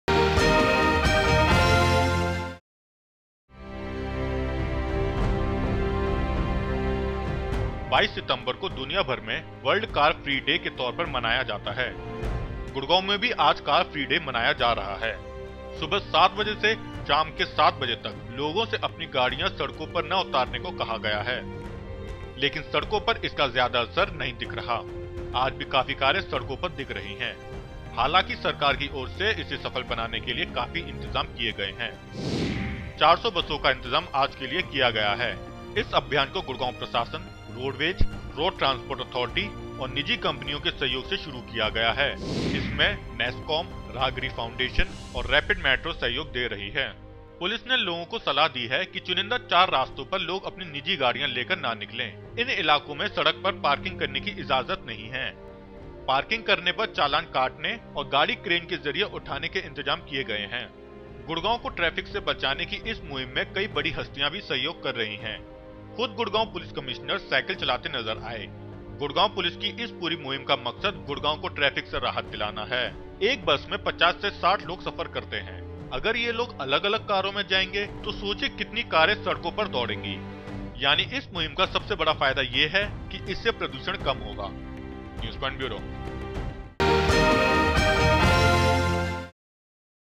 Çar Free Day in Gurgaon today: NewspointTV
More than 400 buses will be made available for commuters in Gurgaon to get to their workplace on Tuesday, which is being observed as ‘Car Free Day’.

Busy with preparations to ensure the event is a success, organiser Sarika Panda Bhatt, manager (city and transport) with Embarq (India), said every effort is being made to ensure commuters are not stranded.

“Both government agencies and corporate houses have pledged support in terms of more buses for commuters on D-day. The Secretary (Road Transport Authority) and the Haryana Roadways are contributing 100 and 90 buses each, Shuttl.com will run 150 buses. More than 400 buses will be on the road on Tuesday, which is 250 more than the usual number. Besides, 90 buses run everyday as part of the city bus service as well,” said Ms. Bhatt.

The organisers have also received demands for bicycles from various corporate houses and the Municipal Corporation of Gurgaon (MCG).

“We have been provided 50 bicycles by Hero and 190 by the MCG to supply to those willing to pedal to work. We had also received demands for cycles from various government and private offices and gave them the same. We decided against keeping bicycles at metro stations as it would have led to a chaotic situation.”

The organisers have also appealed to commuters to opt for car pools or take cycle rickshaws and auto-rickshaws to reach office. Vehicular traffic is expected to be reduced by at least 20 per cent on September 22.